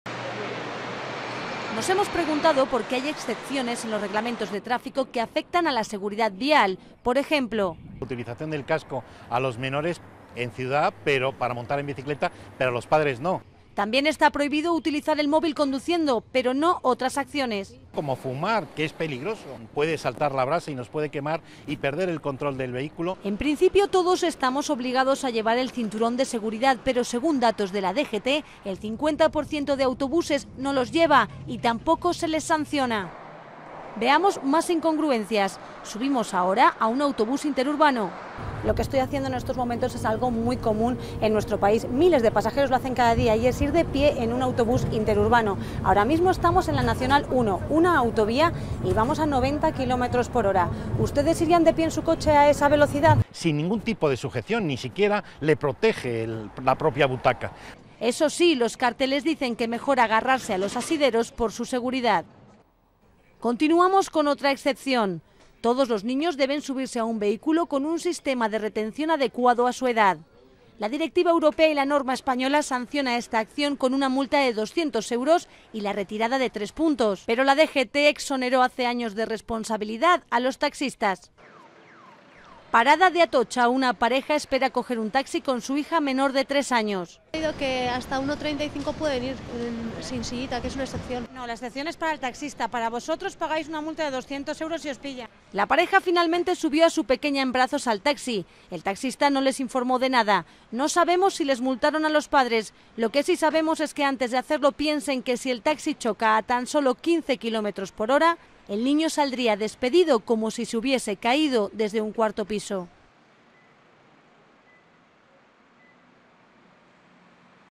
Las excepciones de la seguridad vial
Autobuses sin cinturón y bicis sin casco, algunas conductas no penalizadas por Tráfico